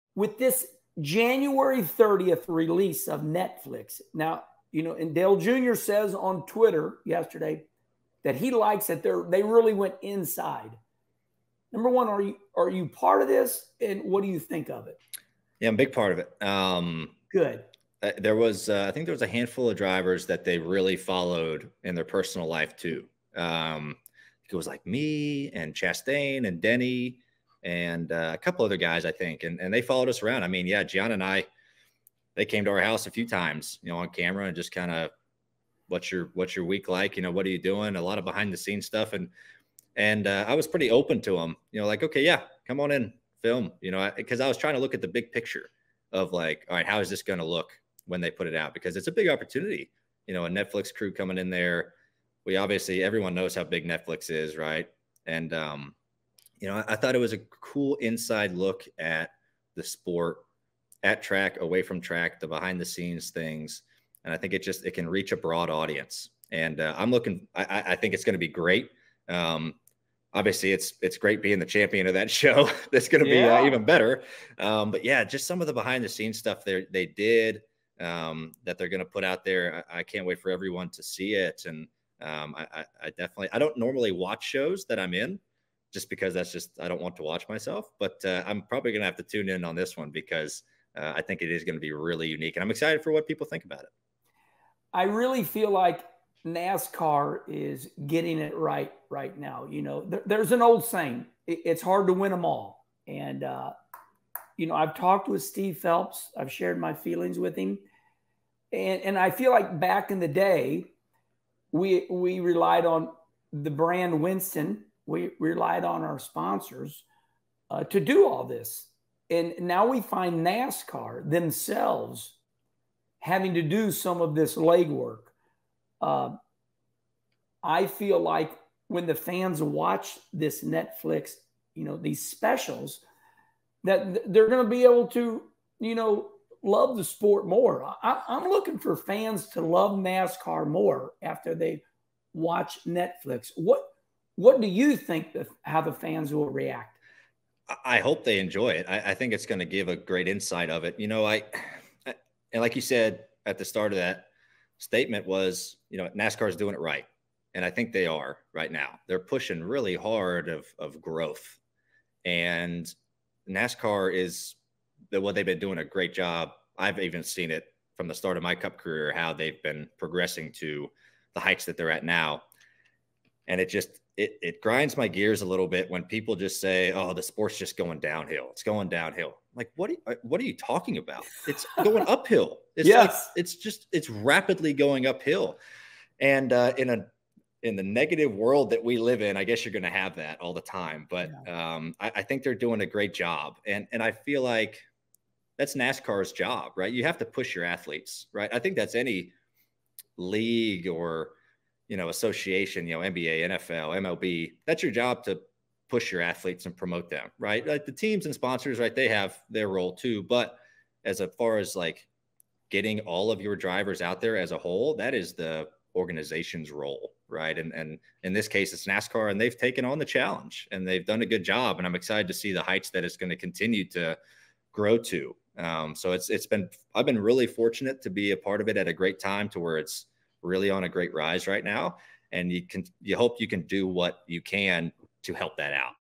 "NASCAR Full Speed" on Netflix is Awesome!
Episode #38 of "Kenny Conversation" is 2023 NASCAR Cup champion Ryan Blaney!

In this clip Blaney discusses his role in "NASCAR Full Speed" on Netflix.

***thumbnail & b-roll photos by Getty Images courtesy of NASCAR Media